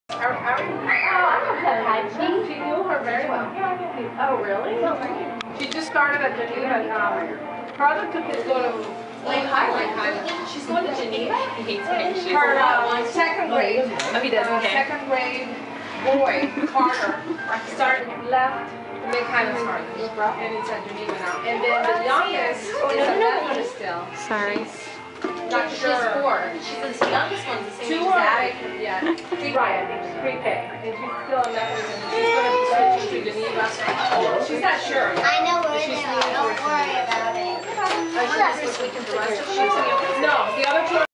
Jan 23, 2012 - Ansley's 2nd ballet class - Ans and E's dance.MP4
Ansley and Eliza's partner dance thing... :)